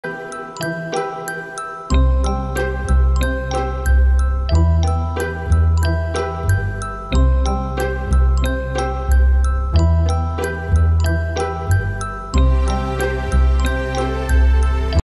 Christmas Candy Dome ornaments, DIY sweet Xmas gift idea, Santa hat, mittens and sacks.
Santa Ornament Christmas Candy Dome using SVG digital files, card stocks, plastic dome(hall of Xmas fillable baubles) and cutting plotter.